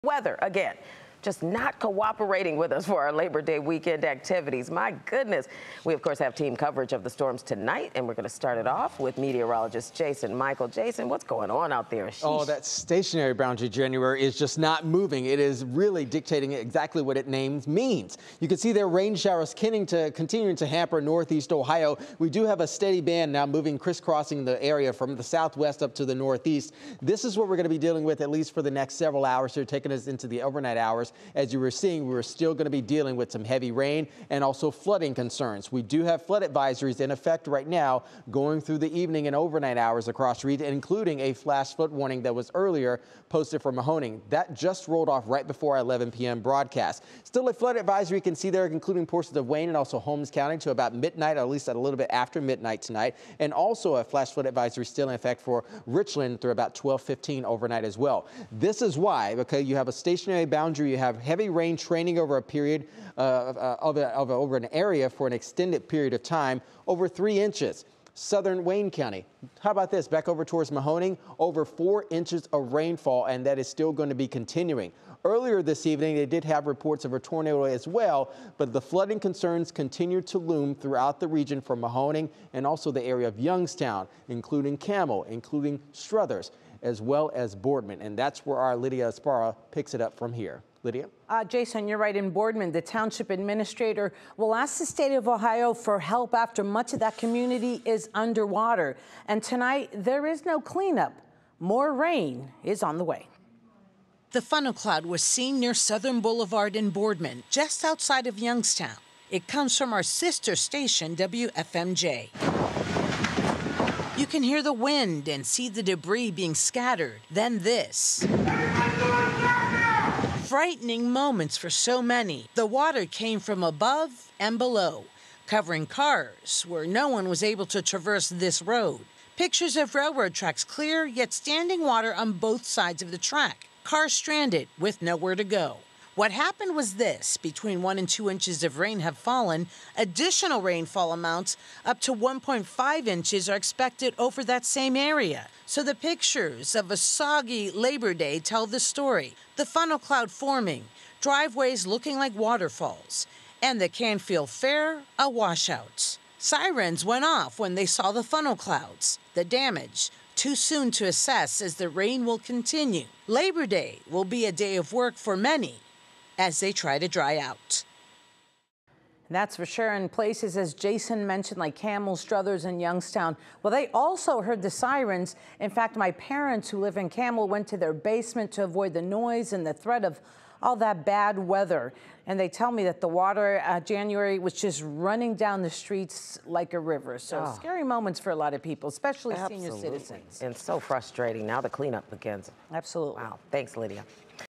Severe weather struck parts of the Northeast Ohio region Sunday with flooding and a possible tornado
Several areas in the Northeast Ohio region were under flood advisories and warnings at various points Sunday. The weather even caused several Labor Day events to cancel plans for the day.

3News' Lydia Esparra assesses the damage in Boardman as flooding and heavy winds ripped through the area causing a massive amount of damage in the state's Northeast region.